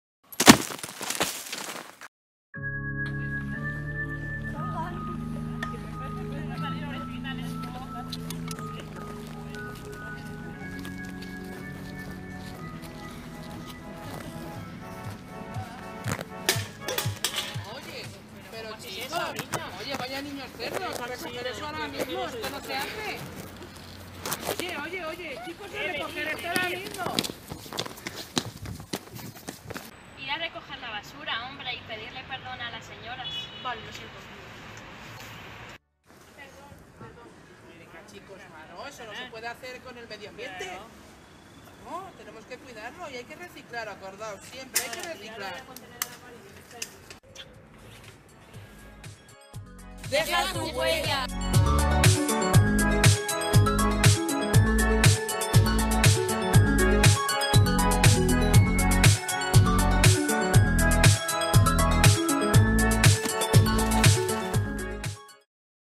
Deja tu huella
Hemos querido contribuir con esta grabación a mejorar el medio ambiente con pequeñas acciones.

Santa María de la Expectación
Festival de Clipmetrajes de Manos Unidas